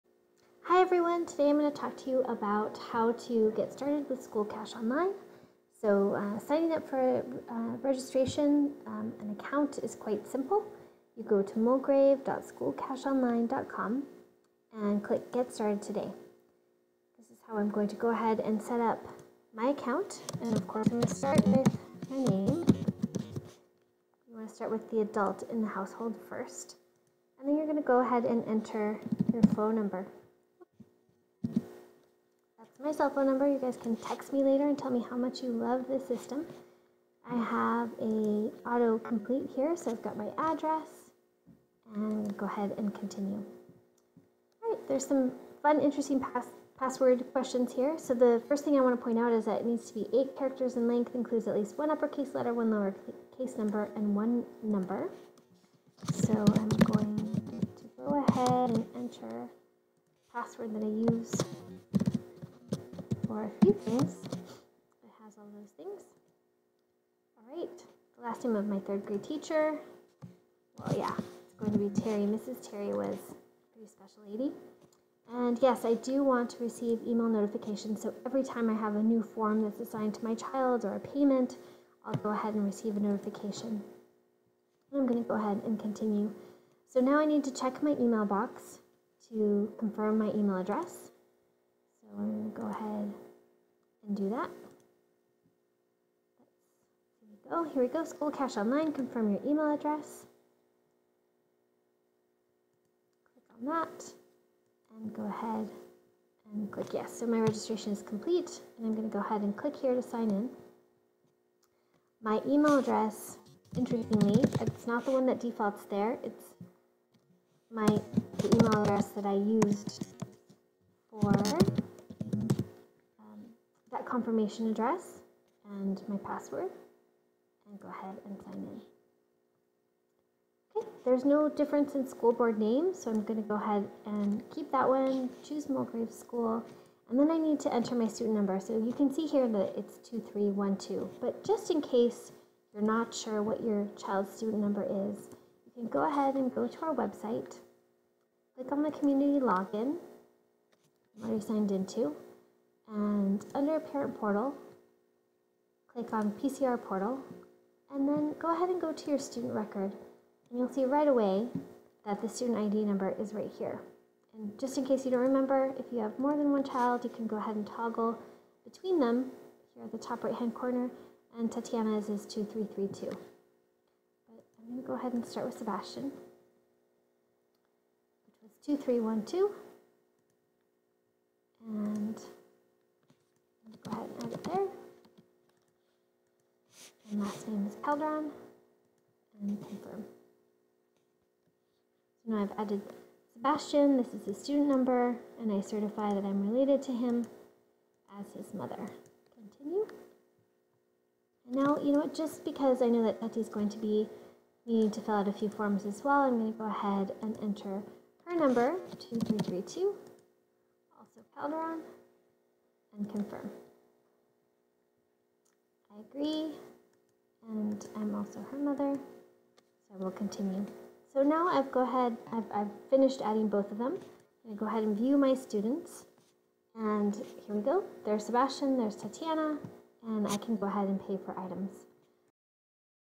School Cash Online Registration
A how-to video for registation of students by ID number